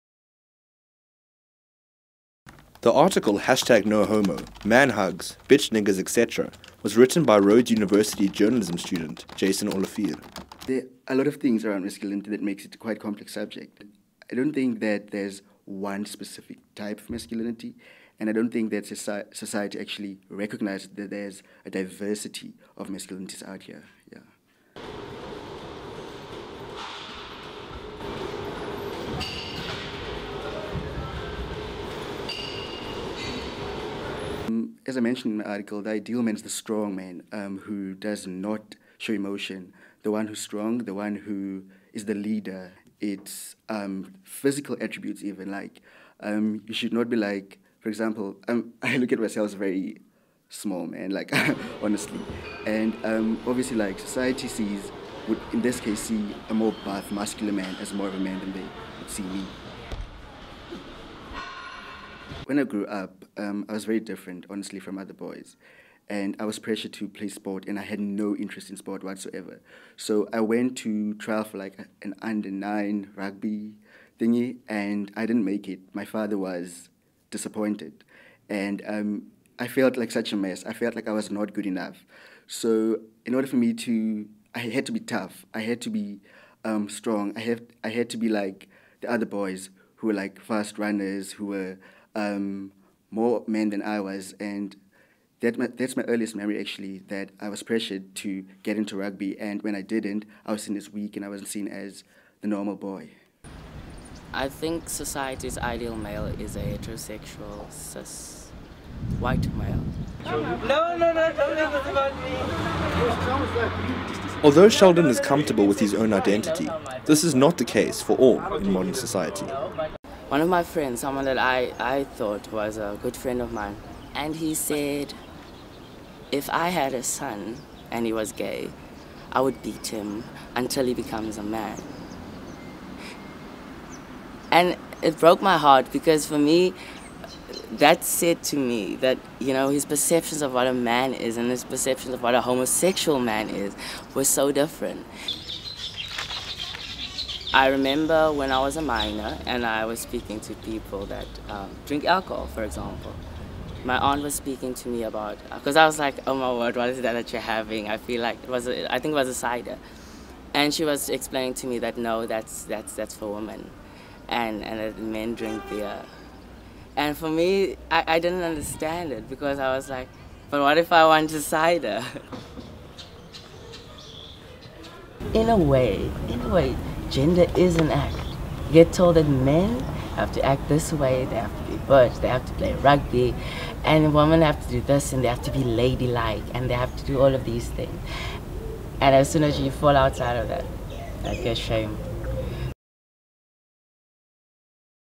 Ideas of Masculinity
Rhodes University students Jason Olivier and Sheldan Dolf   challenge societal perceptions and norms around masculinity. We explore the challenges they have face in confronting these norms. Olivier and Dolf both discuss issues such as sport, physical size, family reactions and even alcohol preference.

Production by Nic Andersen, Dan Tucker,  Abner Accom